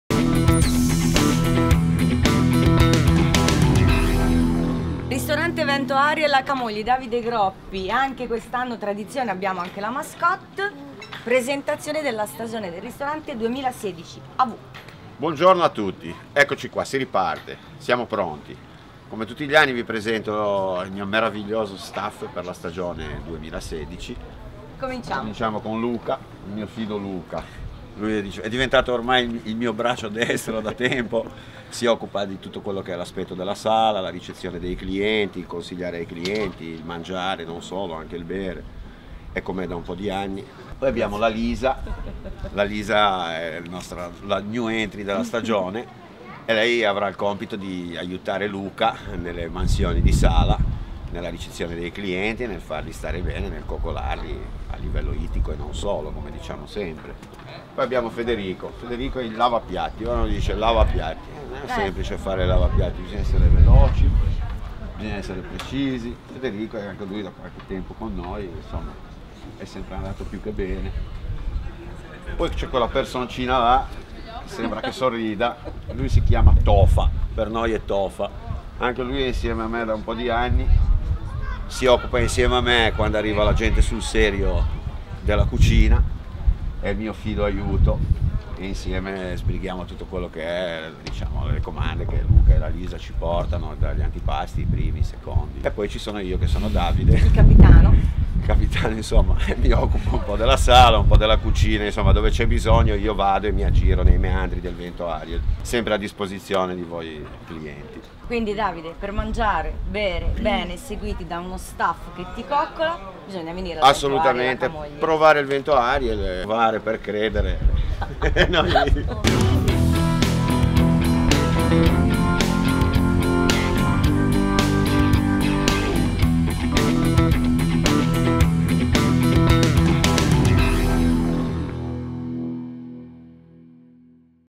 Coccole a livello ittico al Ristorante Vento Ariel di Camogli
Coccole a livello ittico al Ristorante Vento Ariel di Camogli. Provare per credere!
Davide Groppi, chef del Ristorante Vento Ariel di Camogli, proprio sul porticciolo dove attraccano i pescatori per portargli il pesce fresco a miglio zero, ci presenta il suo staff per la stagione 2016!
Pesce fresco cucinato alla ligure o rivisitato per le preferenze dei clienti provenienti da tutto il mondo e una cantina vini di tutto rispetto con etichette liguri, italiane e anche straniere.
Se aspirate a coccole a livello ittico, Vento Ariel, provare per credere!
SOCIAL SU:
PER IL TUO VIDEO PUBLIREDAZIONALE SU ZENAZONE